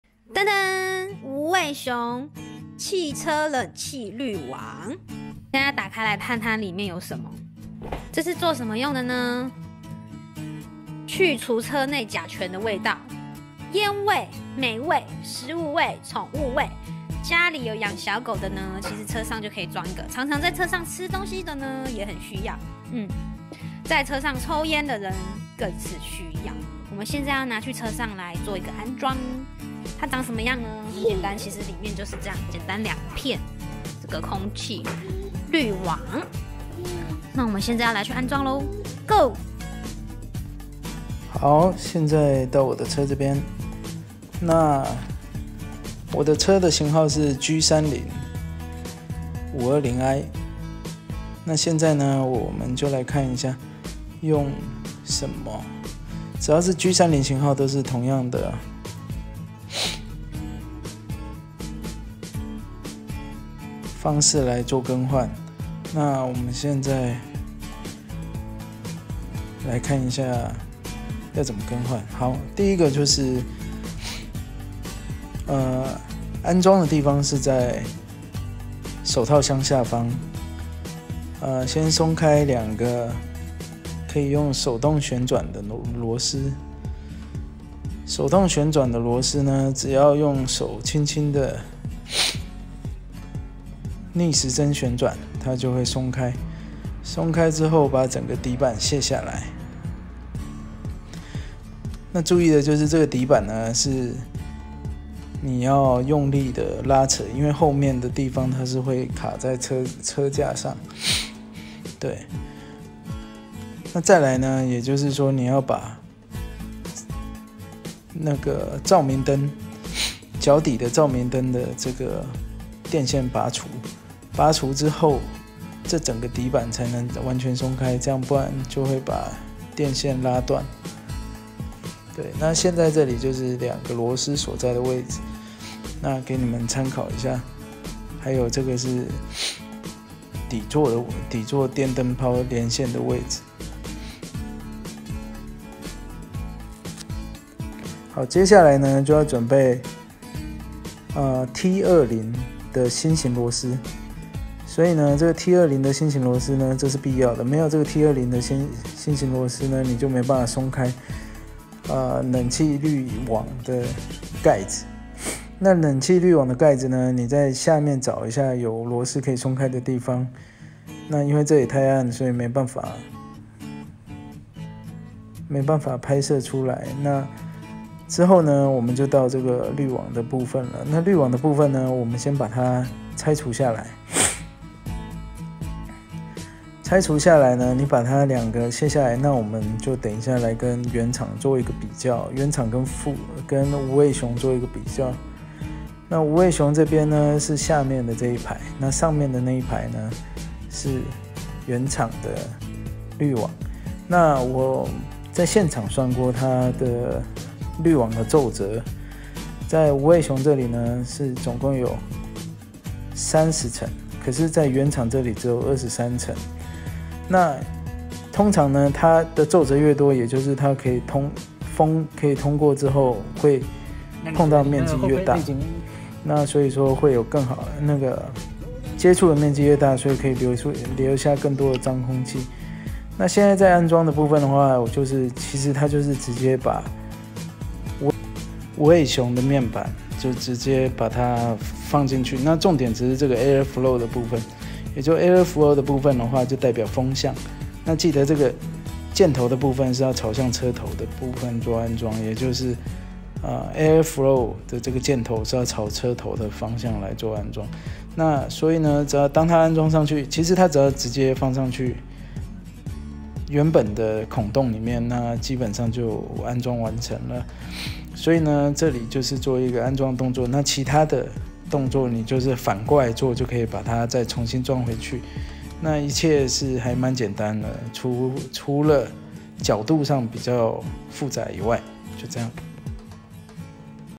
BMW G30 冷氣濾網更換 DIY 無味熊 bmw520i｜羅好吃Vlog
這隻影片難得是Bo爸當主角，
因為要換汽車冷氣的濾網！
我只能幫忙拍囉！

 您好！我是羅好吃也是Bo媽！
一個女兒的媽咪～女兒叫BoBo 所以變成Bo媽
喜歡推薦好物，吃喝玩樂樣樣推